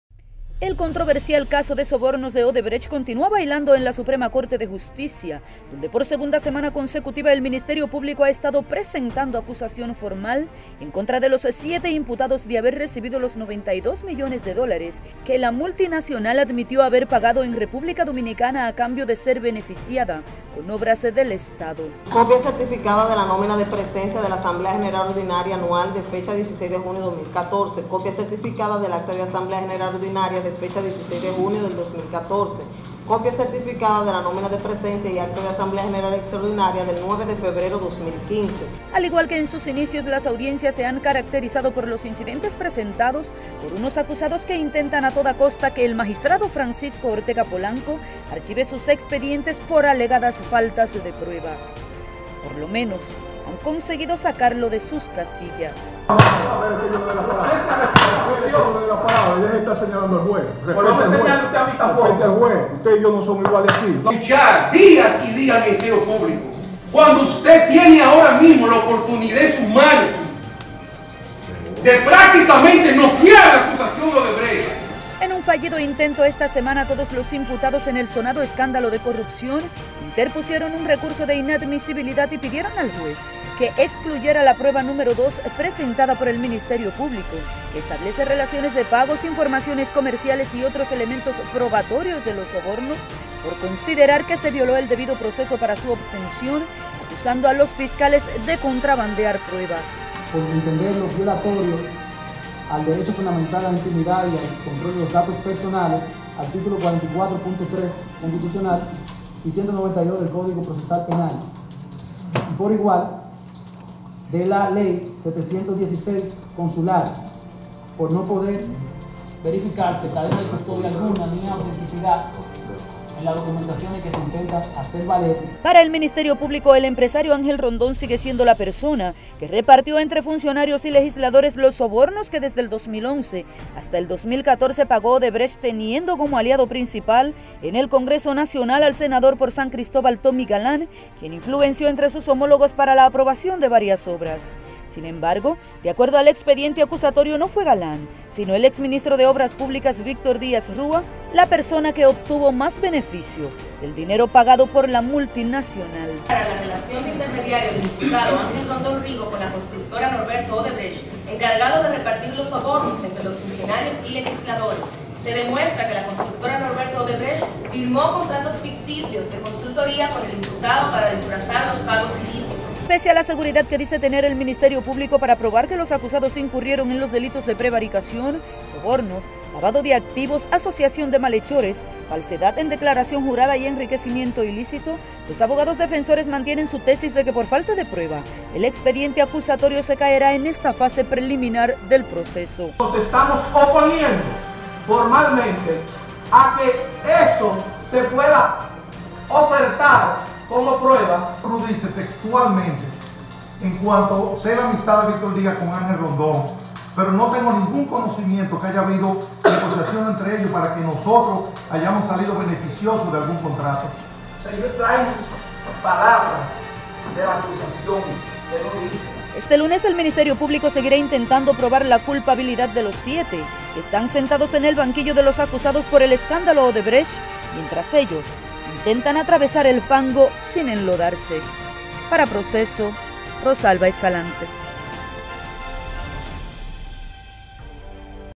ODEBRECHT EN TRIBUNALES POR ROSALBA ESCALANTE - PROCESO 3 02 2019
El controversial caso de sobornos de Odebrecht continua bailando en el Tribunal de Justicia, donde por segunda semana consecutiva el Ministerio Público ha estado presentando acusaciones formal en contra de los siete imputados de haber recibido los 92 Millones de dólares que la multinacional admitió  haber pagado a la República Dominicana a cambio de ser beneficiada con obras del estado.